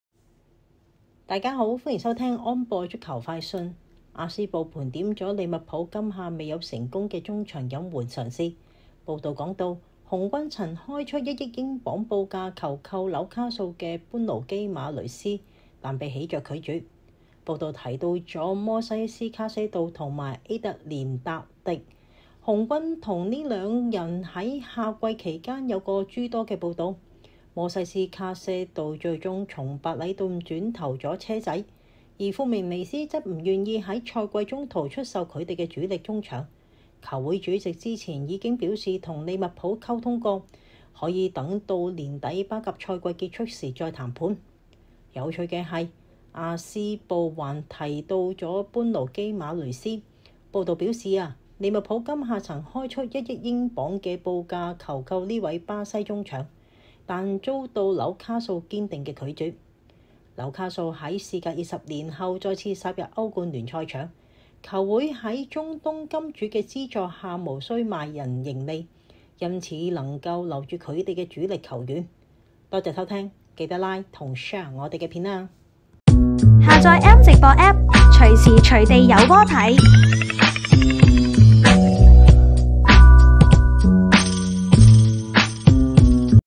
【英超-最新消息】2023-09-09 紅軍曾1億報價基馬雷斯 [聲音報導: Jojo]
《阿斯報》盤點了#利物浦 今夏沒有成功的中場引援嘗試，報導稱，紅軍曾開出1億英鎊報價求購紐卡素的般奴.基馬雷斯 ，但被喜鵲拒絕。英超

報導提到了摩西斯.卡些度和A.特連達迪，紅軍與這兩人在夏季期間有過諸多報導，摩西斯.卡些度最終從白禮頓轉投了車仔，而富明尼斯則不願意在賽季中途出售他們的主力中場，球會主席此前已表示和利物浦溝通過，可以等到年底巴甲賽季結束時再談判。

有趣的是，《阿斯報》還提到了般奴.基馬雷斯。報導表示，利物浦今夏曾開出1億英鎊的報價求購這位巴西中場，但遭到紐卡素堅定地拒絕。

紐卡素在時隔20年後再次殺入歐冠聯賽場，球會在中東金主的資助下無需賣人盈利，因此能夠留住他們的主力球員。